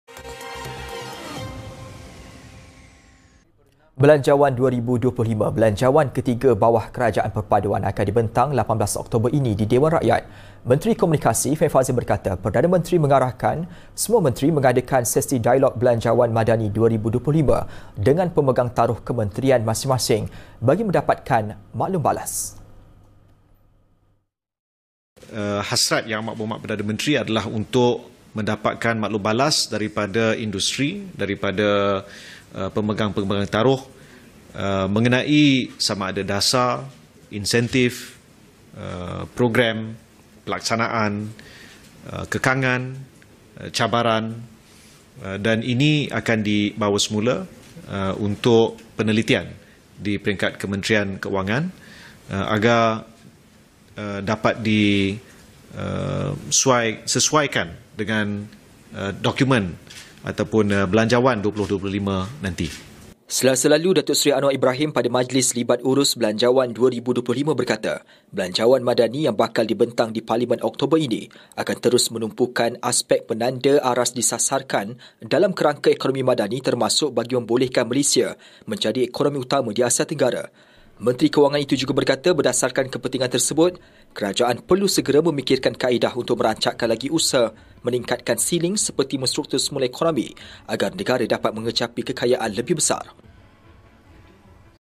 15 JULAI 2024 | Belanjawan 2025 dibentang 18 Okt ini
Belanjawan 2025, belanjawan ketiga bawah Kerajaan Perpaduan, akan dibentang 18 Oktober ini di Dewan Rakyat.